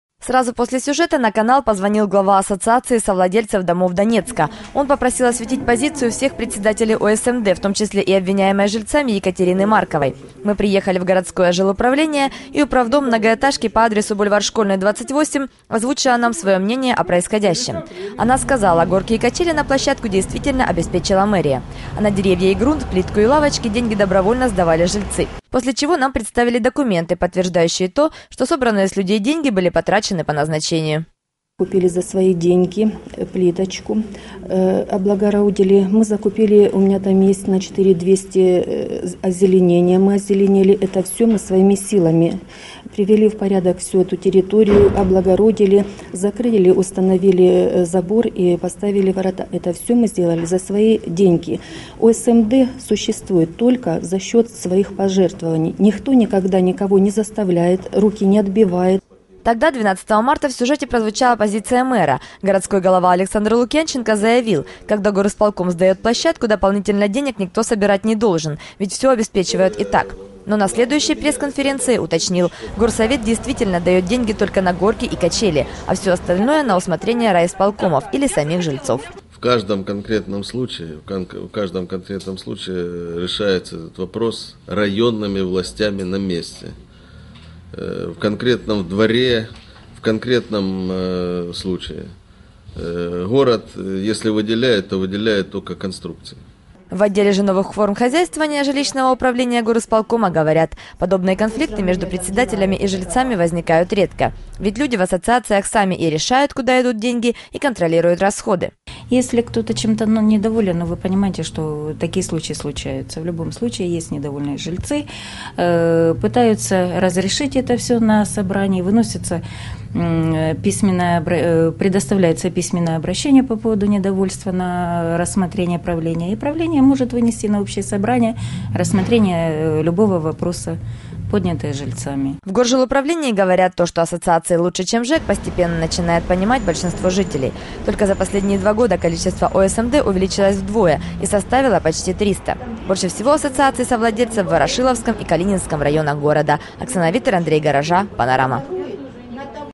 ОСМД
Председатель ОСМД на бульваре Школьном тратит деньги жильцов по назначению, и поборов в доме 28  нет!